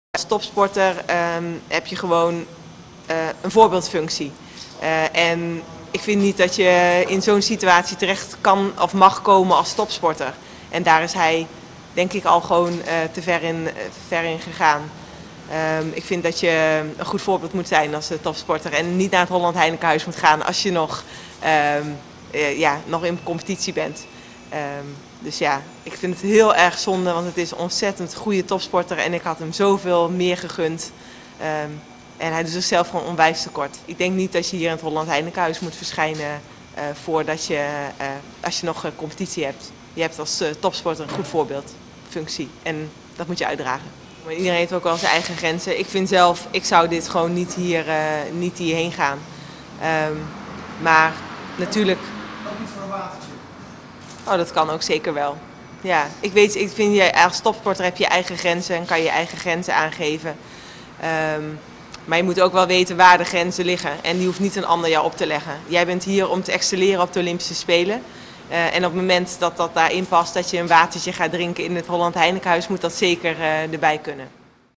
Annemiek reageert op uitspraak Yuri
RIO DE JANEIRO - Wielrenster Annemiek van Vleuten vindt eigenlijk dat je als topsporter op de Olympische Spelen niet in het Holland Heineken House moet komen.Natuurlijk mag je er een watertje drinken, maar ze benadrukt dat je als topsporter een voorbeeldfunctie hebt en het gebruik van alcohol is niet passend als je op een wereldpodium bezig bent om te excelleren.